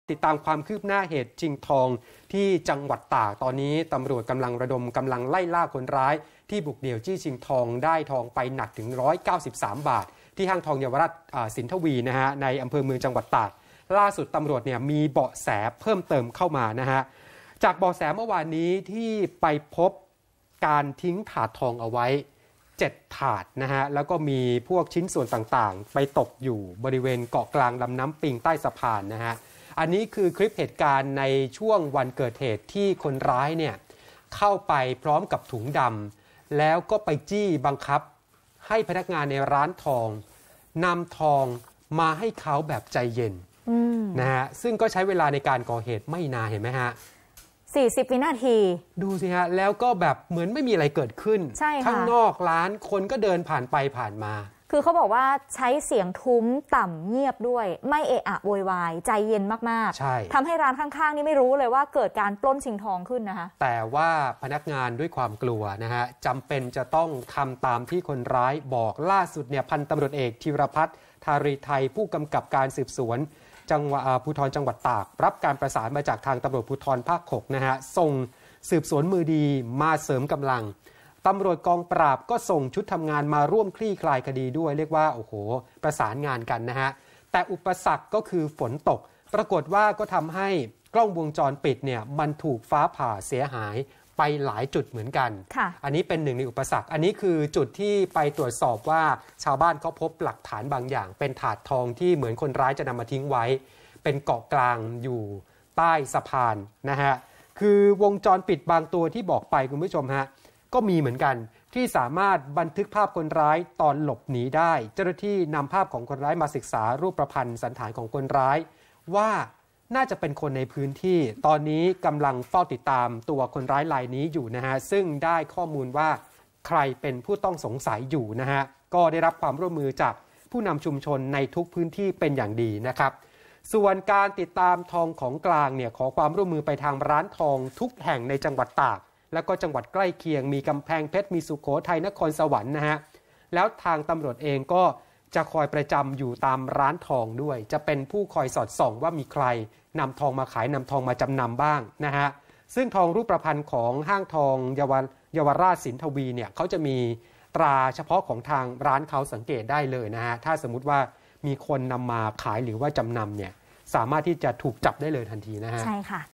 ระดม ตร.สืบภาค 6–กองปราบฯ ล่าตัวคนร้ายชิงทอง
ตำรวจระดมกำลัง ล่าคนร้ายบุกเดี่ยวจี้ชิงทอง หนัก 193 บาท ในห้างทองเยาวราช สินทวี ในห้างสรรพสินค้า อ.เมือง จ.ตาก ล่าสุดได้เบาะแสเพิ่ม คาดเป็นคนในพื้นที่